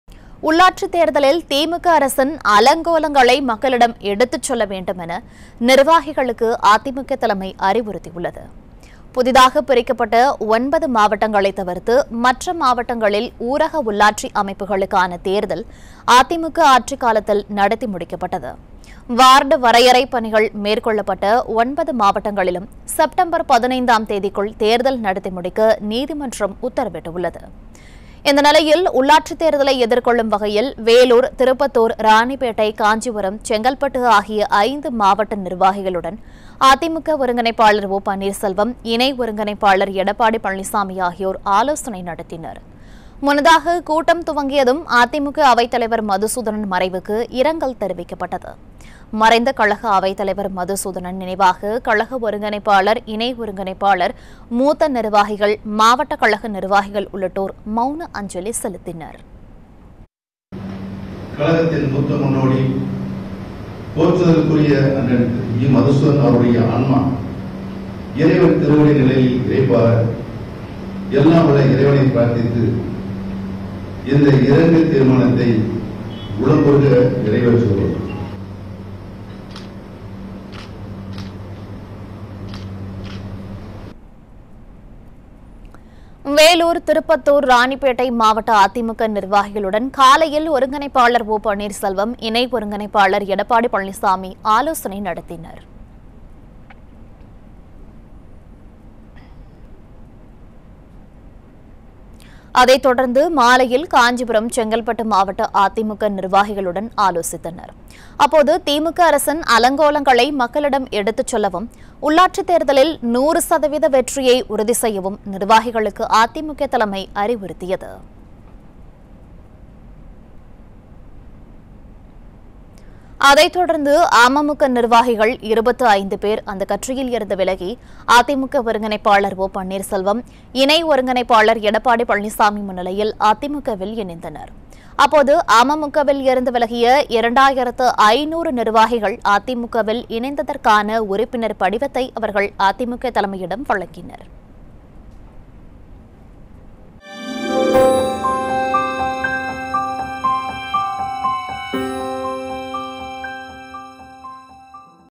"உள்ளாட்சி தேர்தலில் திமுக அரசின் அலங்கோலங்களை மக்களிடம் எடுத்து சொல்ல வேண்டும்"
"உள்ளாட்சி தேர்தலில் திமுக அரசின் அலங்கோலங்களை மக்களிடம் எடுத்து சொல்ல வேண்டும்" - அதிமுக தலைமை